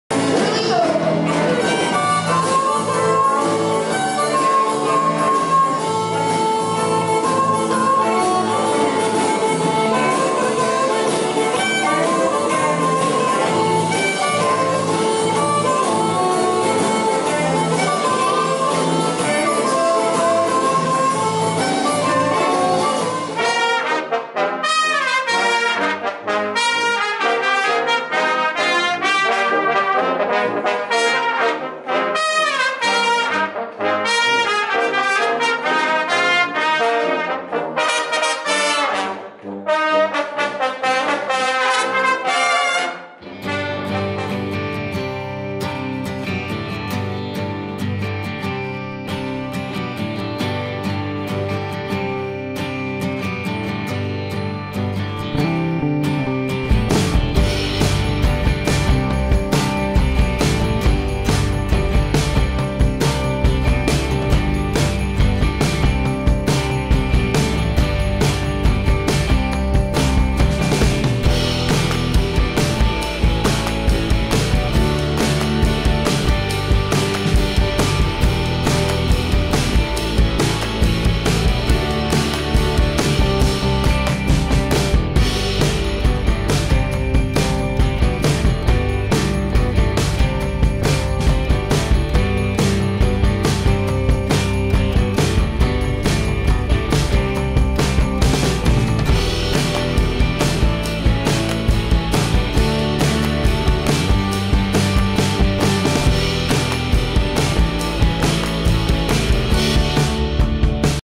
Freeland, Pa has 140th birthday Sept 11, 2016 with Organized Resistance & Tin Roof Brass Band
Freeland, Pa is celebrating its 140th birthday on Sept 11, 2016.  Bands included are Organized Resistance and the Tin Roof Brass Band.  Veterans were Honored and there was Food, music, fun, BBQ

DD Explores
Hazleton, Pa.  18201